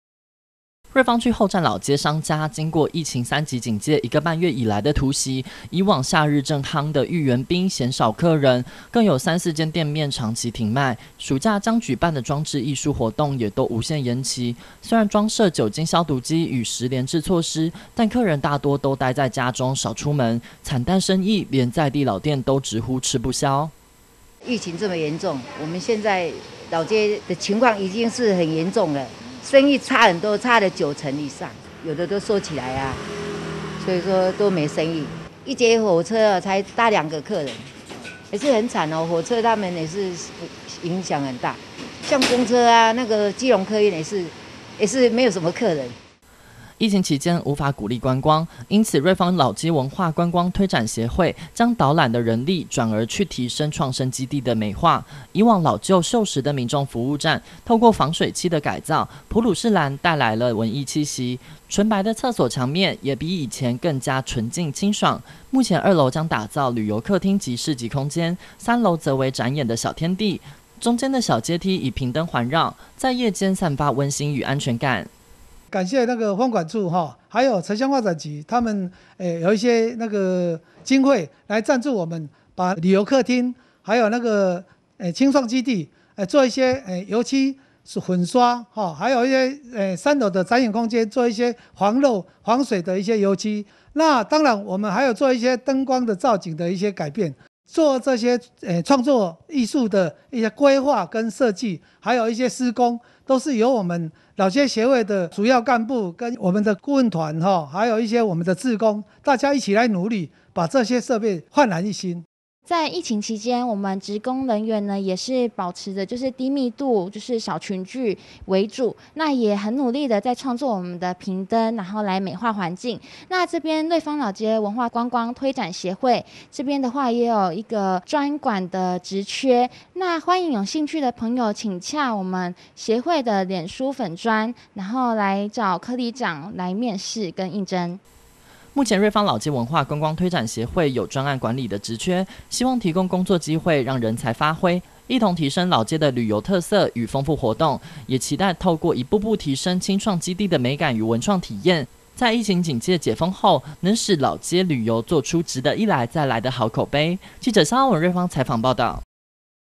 20210701觀天下新聞 新北市瑞芳區後站商家疫情衝擊大 老街協會加強硬體美化
瑞芳火車後站商家在疫情三級警戒時，生意不到一成，而瑞芳老街文化觀光推展協會因為沒遊客導覽，將重心轉往硬體美化，將火車站旁的青創基地重新上漆、裝聚光燈，打造「旅遊客廳」的氛圍，並製作瓶燈燈海，使老舊階梯呈現文藝樣貌，待疫情解封，讓遊客有難忘的參觀體驗。